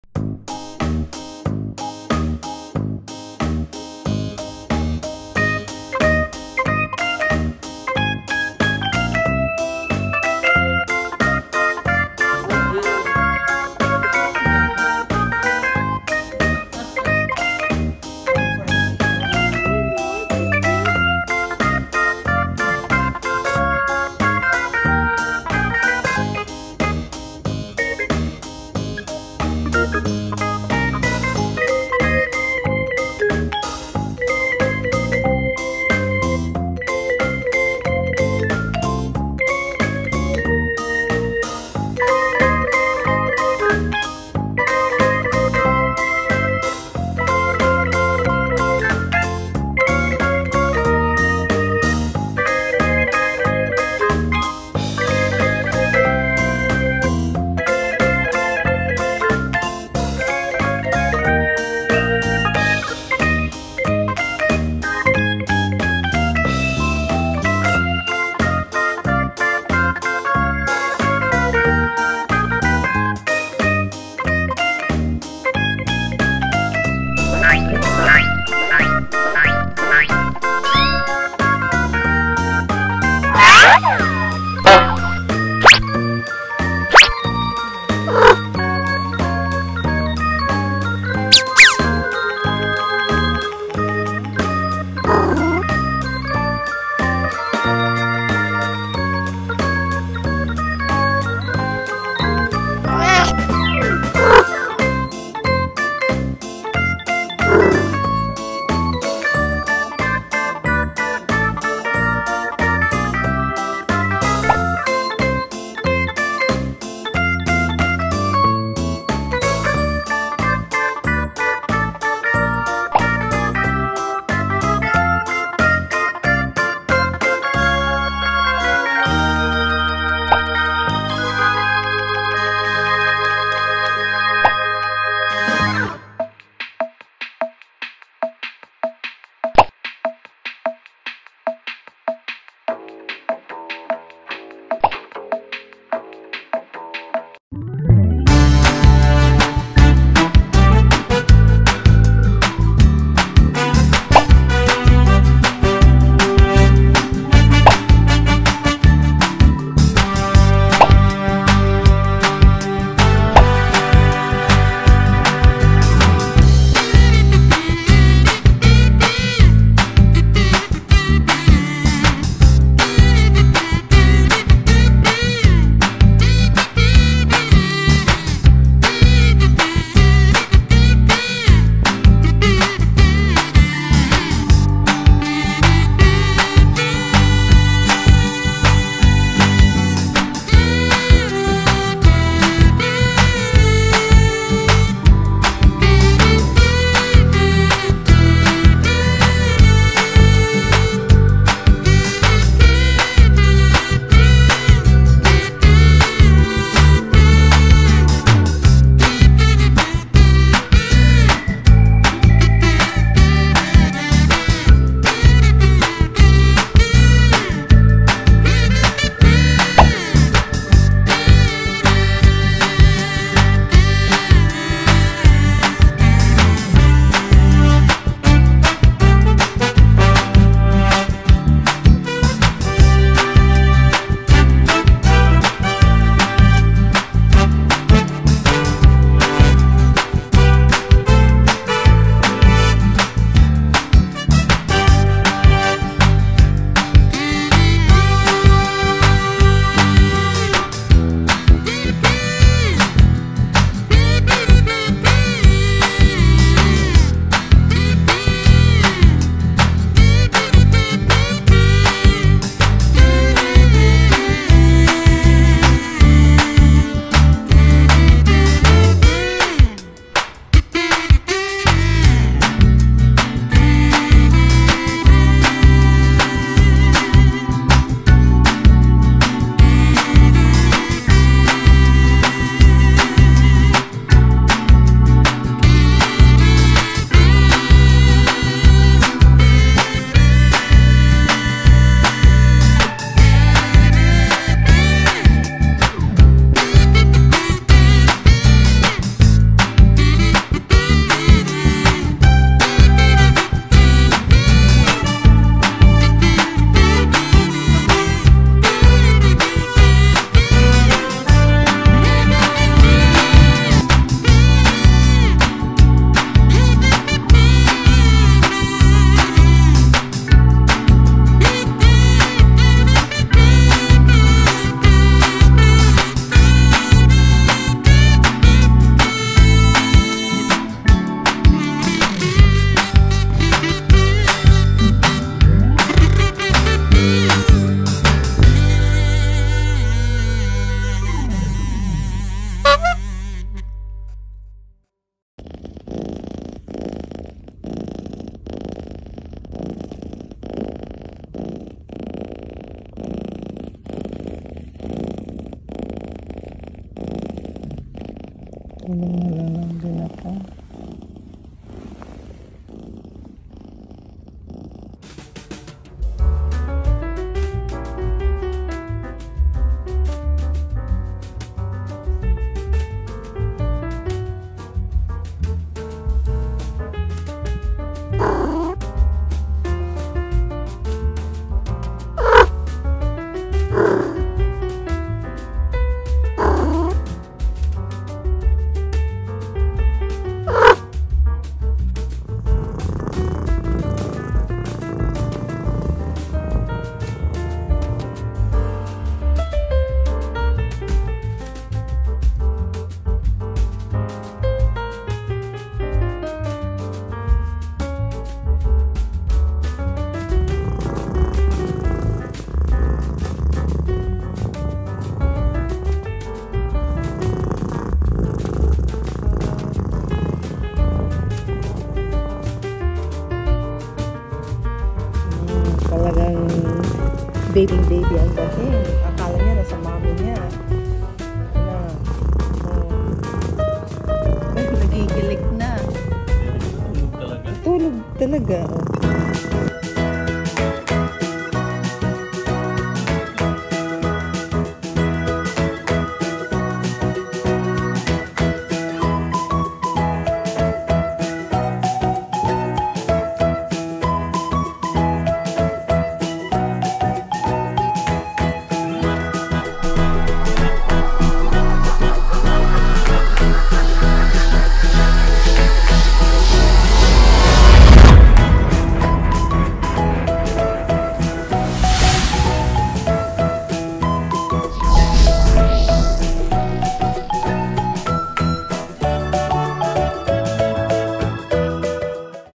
CAT MASSEUR
Have you seen a cat who loves to give really long massages? check out this video